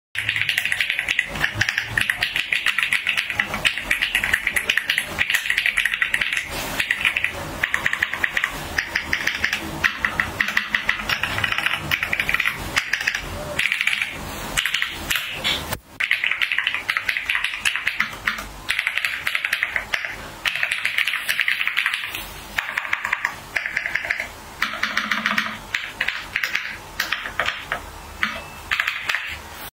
The typing sounds of the Ajazz AK820PRO two linear switches, Flying Fish Switch and Gift Switch👀
🐟Flying Fish Switch:
Total Trip 3.6mm±0.2mm
Trigger Stroke 2.0mm±0.4mm
Trigger Grams 45g±8g
Bottoming Grams 60g±8g

🎁Gift Switch:
Total Trip 3.6mm±0.4mm
Trigger Stroke 2.0mm±0.4mm
Trigger Grams 42g±10g
Bottoming Grams 48g±10g